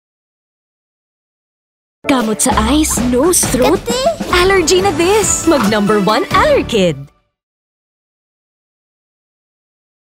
Kamot sa eyes, nose, o makating throat? Alarm na this! Allergy na this! Allerkid na this!
Mommy, pag may kamot sa eyes, nose o makating throat sabay paulit-ulit na hatsing at singhot, AlarmNaThis! Allergy na this, kaya dapat Cetirizine Dihydrochloride (Allerkid) na Fast-acting (Works in as fast as 20 mins.), All-day Ginhawa!

ASC No. U0083P032024A